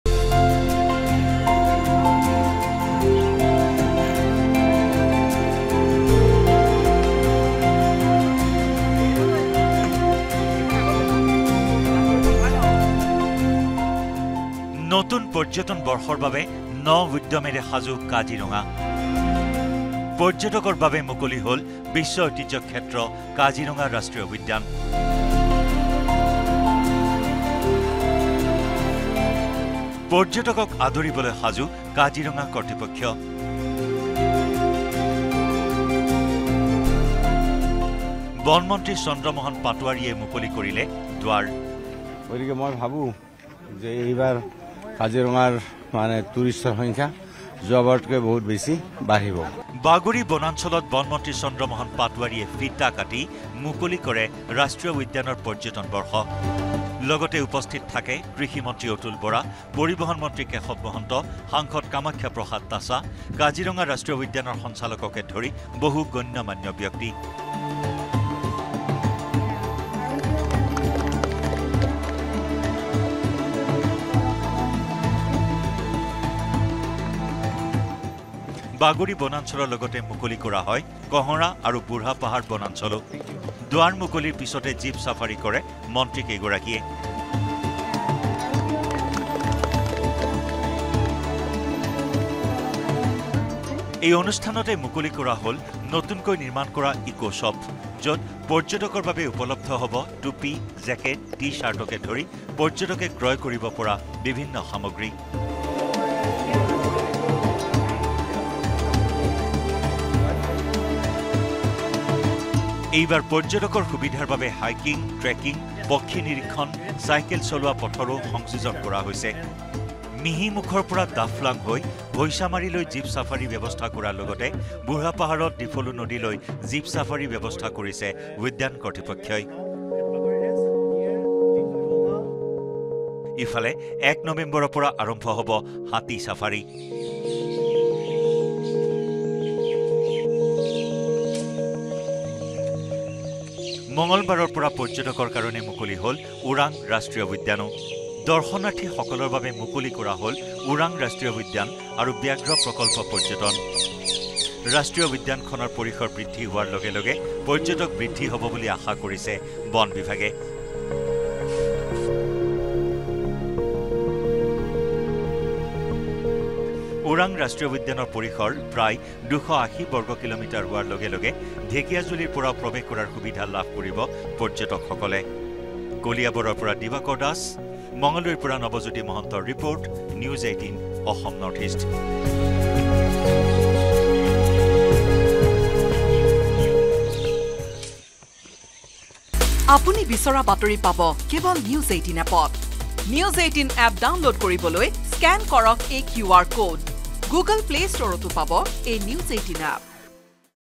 Kaziranga National Park Reopens : নতুন পৰ্যটন বৰ্ষৰ বাবে ন উদ্যমেৰে সাজু কাজিৰঙা | N18V
Kaziranga National Park Reopens | নতুন পৰ্যটন বৰ্ষৰ বাবে ন উদ্যমেৰে সাজু কাজিৰঙা
মুকলি হ’ল বিশ্ব ঐতিহ্য ক্ষেত্ৰ কাজিৰঙা ৰাষ্ট্ৰীয় উদ্যান
পৰ্যটকক আদৰিবলৈ সাজু কাজিৰঙা ৰাষ্ট্ৰীয় উদ্যান
বনমন্ত্ৰী চন্দ্ৰমোহন পাটোৱাৰীয়ে মুকলি কৰিলে উদ্যান